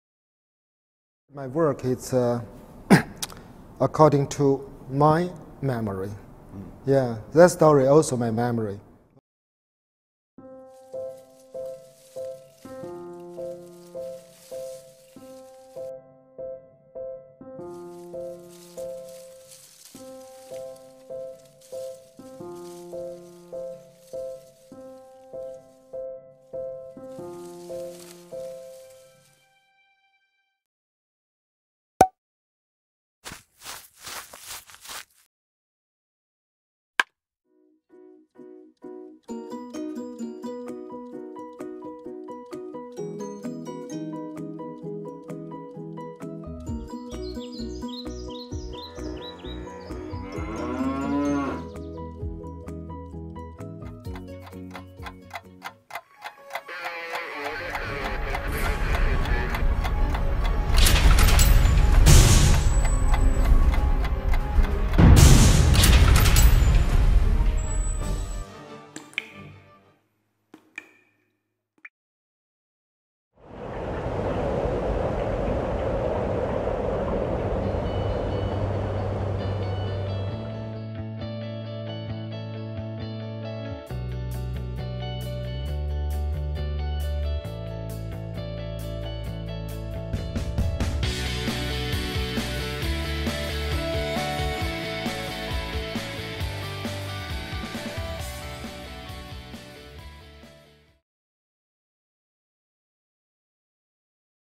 Memory
First-year assignment in university about Architecture and Memory.
Idea came from the interview of architect wangshu and Yeats's poem "Memory".